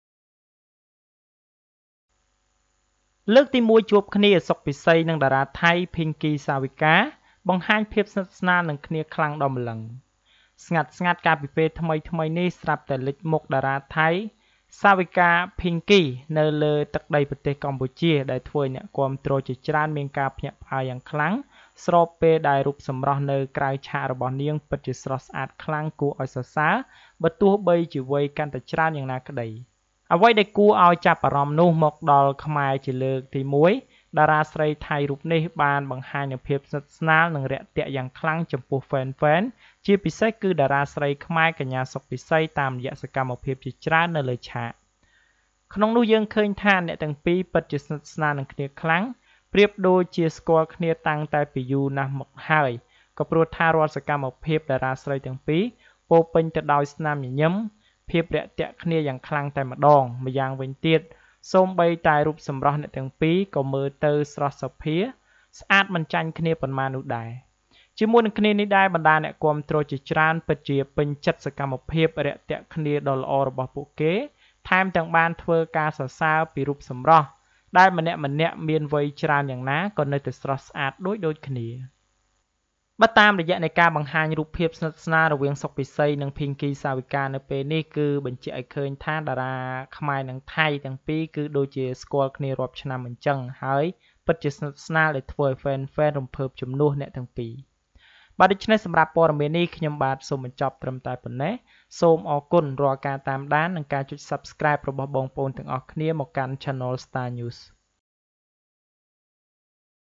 លើកទី១ជួបគ្នា សុខ ពិសី និងតារាថៃ Savika Pinky បង្ហាញភាពស្និទ្ធស្នាលនឹងគ្នាខ្លាំង ដល់ម្លឹង!
I.លើកទី១ជួបគ្នា សុខ ពិសី និងតារាថៃ Savika Pinky បង្ហាញភាពស្និទ្ធស្នាលនឹងគ្នាខ្លាំង ដល់ម្លឹង!